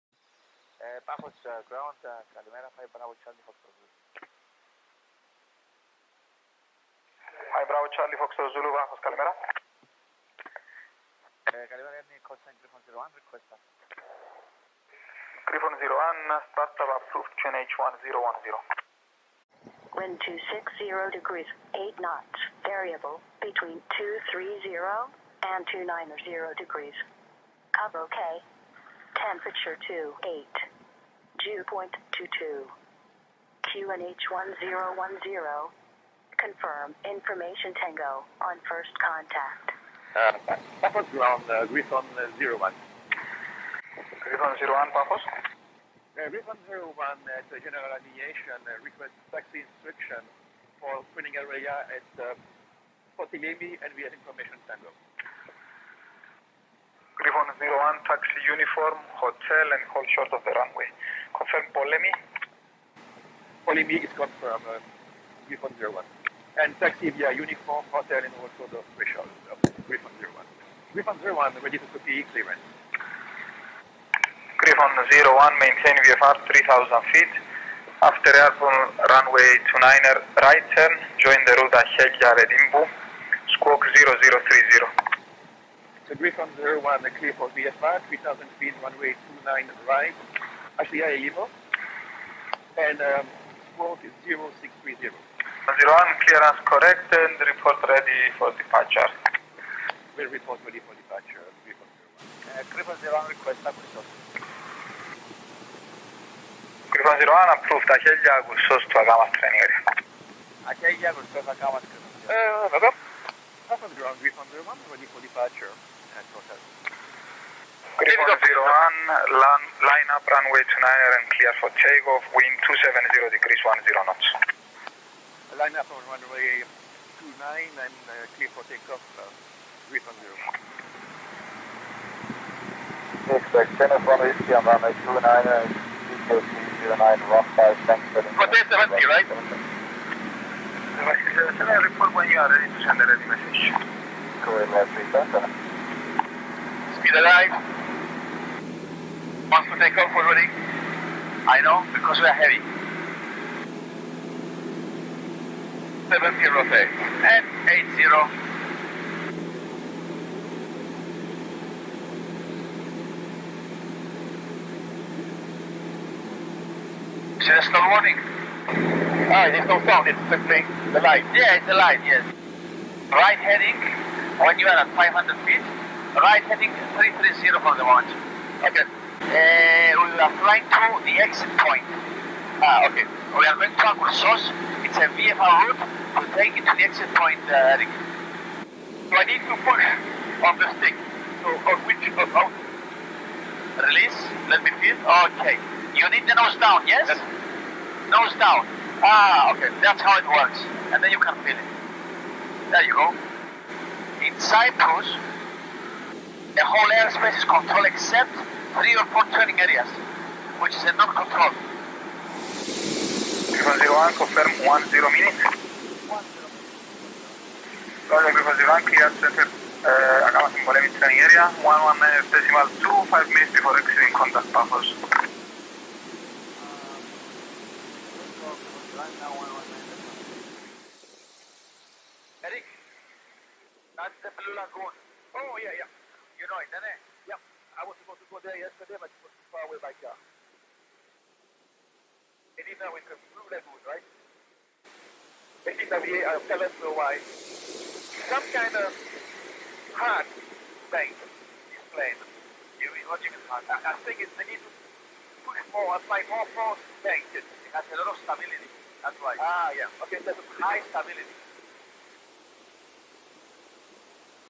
Paphos, Cyprus LCPH departure
All the departure procedures and ATC communication (with transcript) from Paphos General Aviation apron (Griffon Aviation) to the Akamas training area.
A couple of first for me: flying a low-wing plane (PA28 Piper Cherokee), requesting startup clearance, copying departure clearance, ... A real challenge was to pronounce the Greek locations :-)